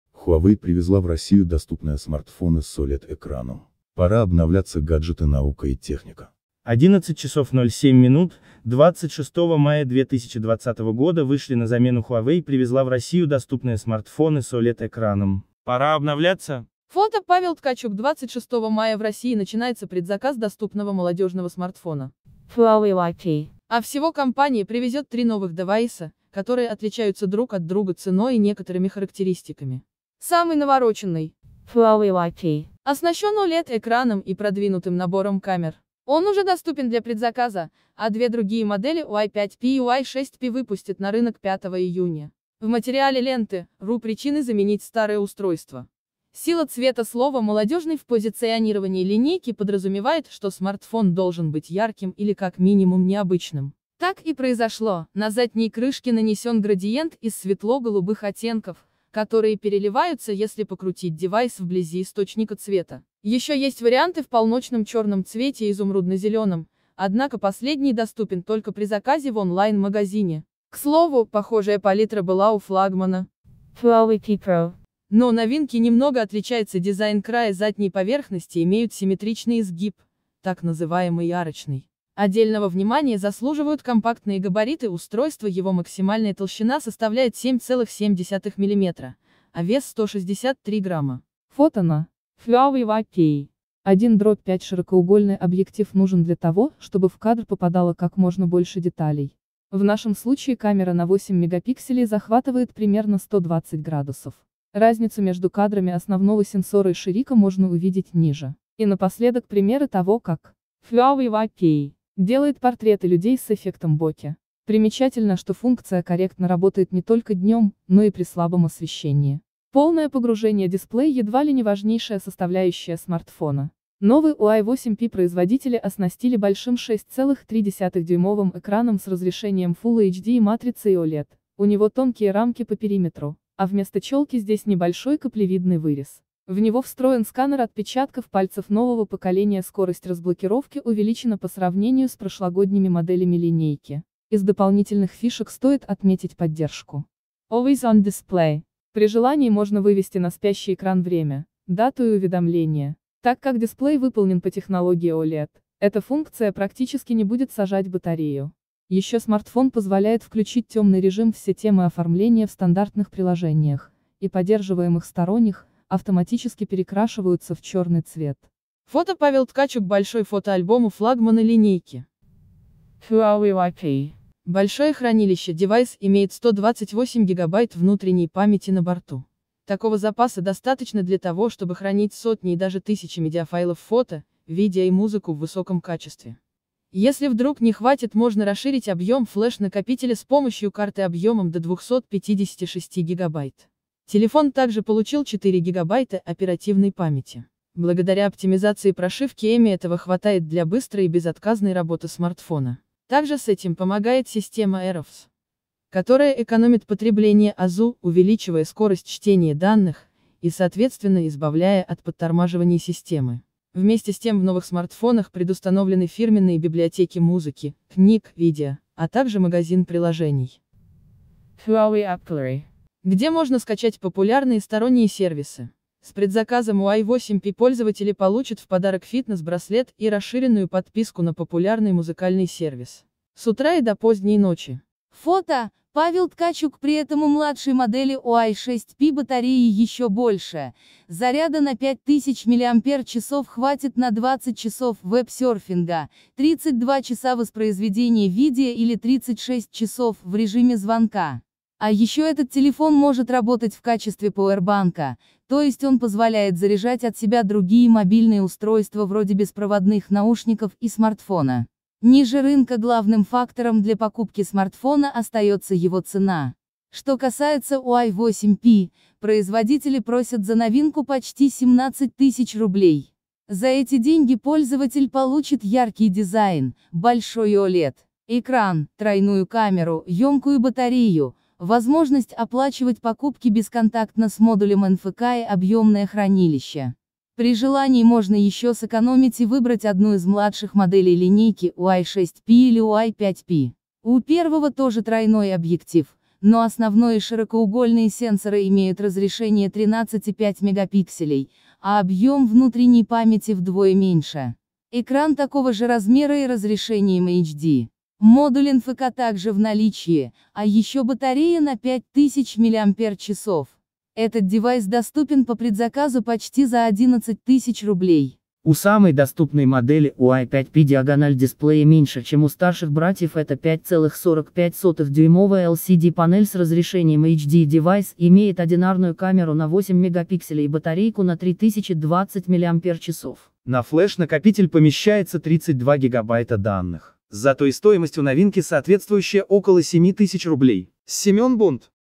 Huawei привезла в Россию доступные с м а р т ф о н ы с OLED-экраном. Пора обновляться
LE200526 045 Huawei привезла в Россию доступные с м а р т ф о н ы с OLED-экраном. Пора обновляться Гаджеты Наука и техника